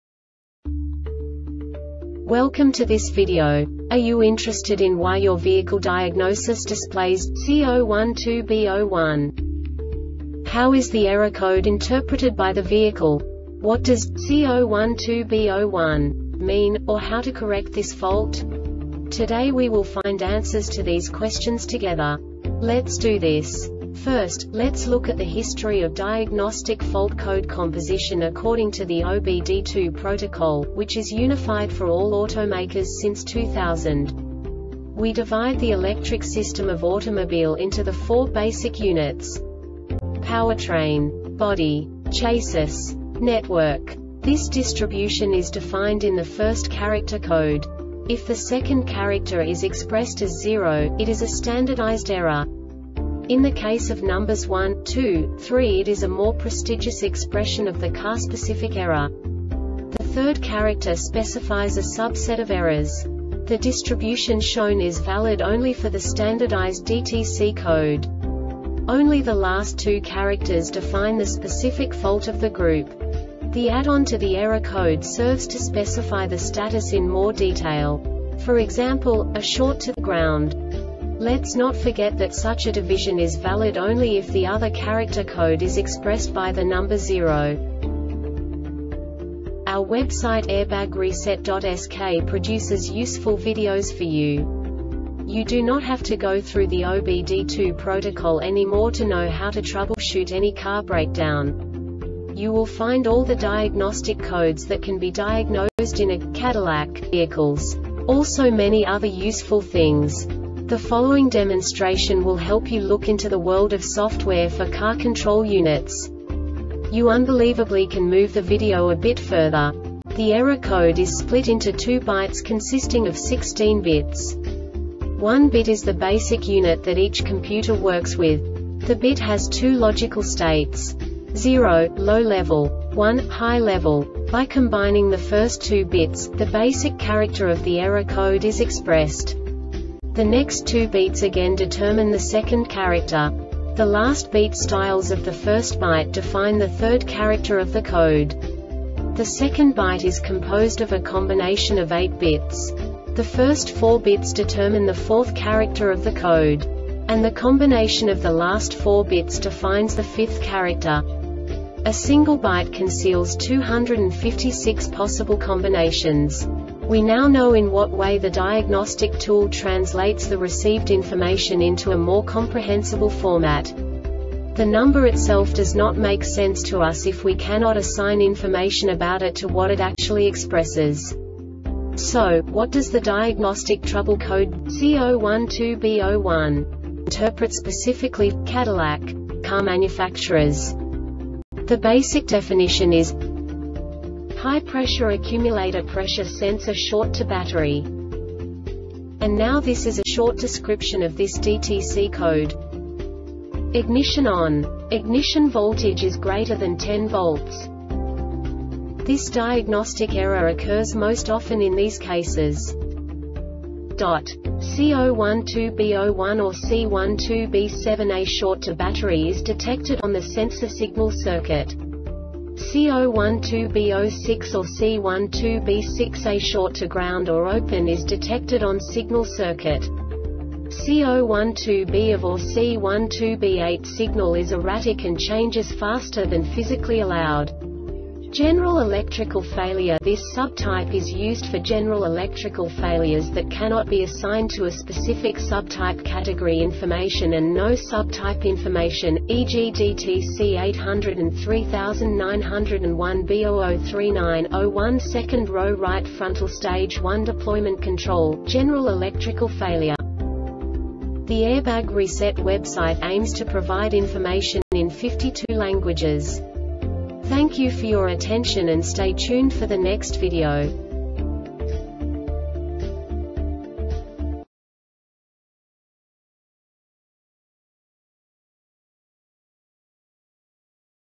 DTC cadillac C012B-01 Short Explanation
Contents:
0:21 Basic DTC analysis according to OBD2 protocol standard.
1:48 Insight into programming
2:58 A diagnostically understandable description of the Diagnostic Trouble Code
3:05 Basic error definition

Make:
Cadillac
Code:
C012B 01
Definition:
High Pressure Accumulator Pressure Sensor Short to Battery
Description:
Cause:
C012B 01 or C12B7
A short to battery is detected on the sensor signal circuit.
C012B 06 or C12B6
A short to ground or open is detected on signal circuit.
C012B OF or C12B8
Signal is erratic and changes faster than physically allowed.